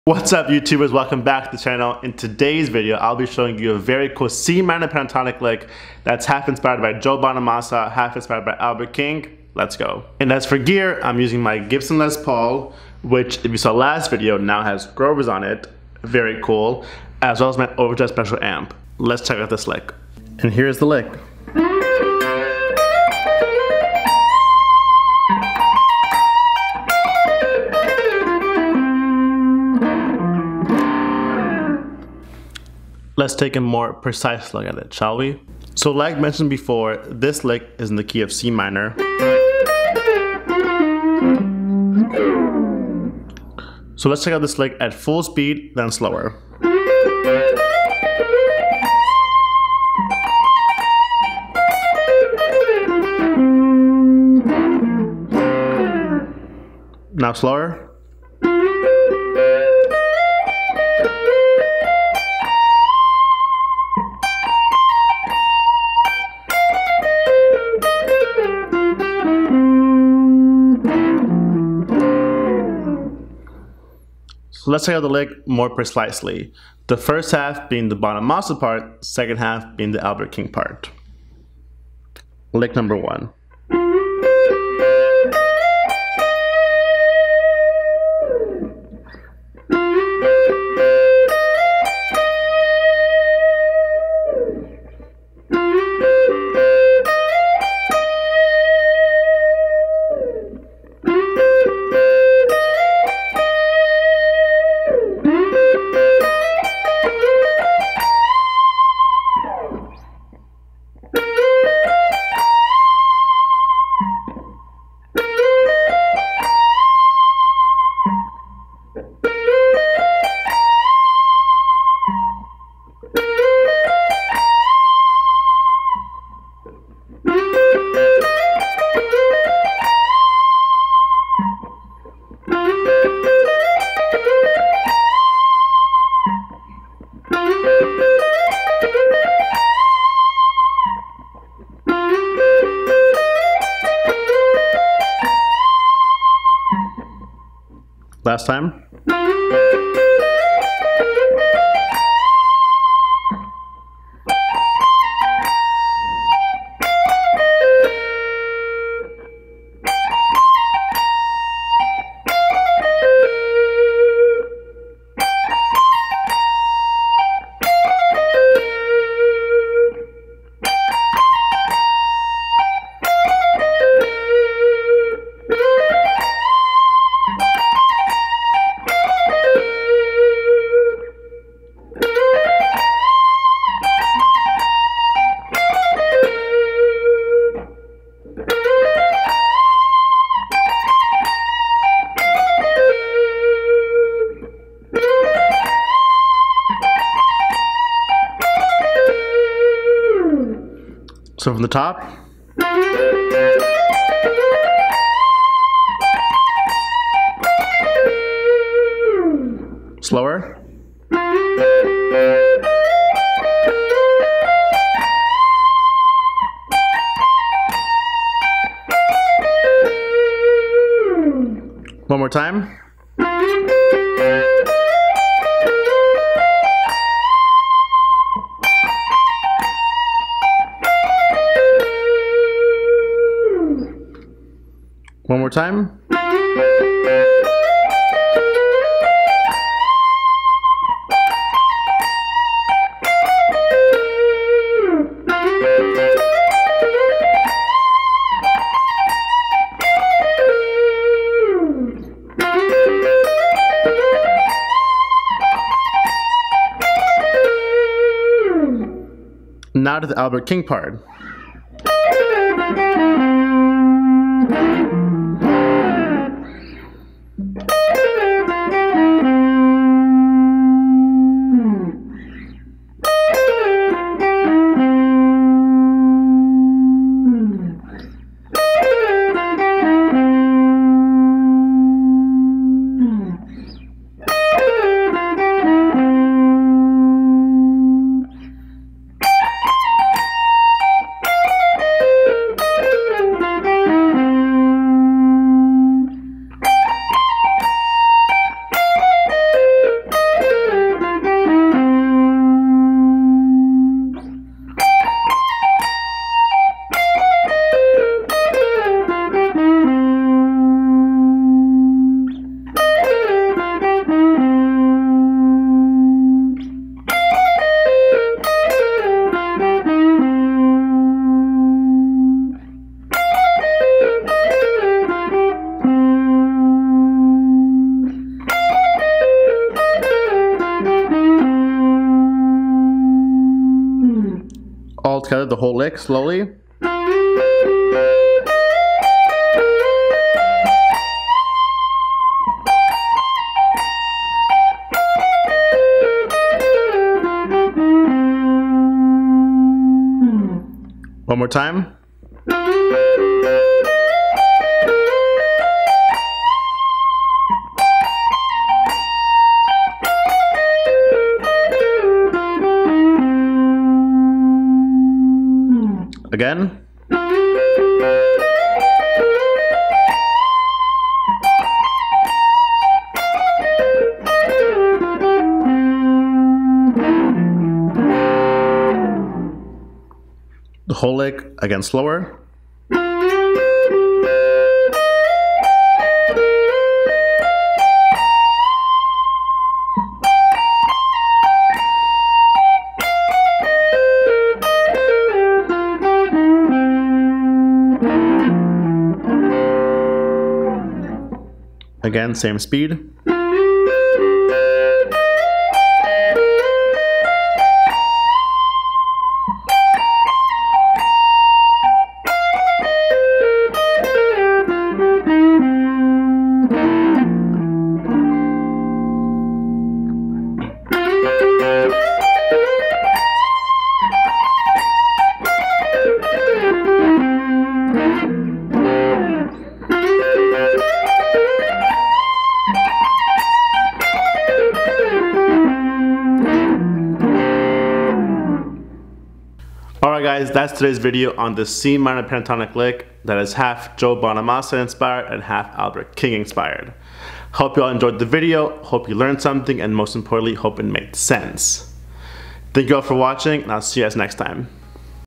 Super Cool Joe Bonamassa/Albert King Lick!
Whats up guys, welcome back to the channel! In todays video, we will look over this C Minor Pentatonic Lick that is half inspired by Joe Bonamassa and half inspired by Albert King.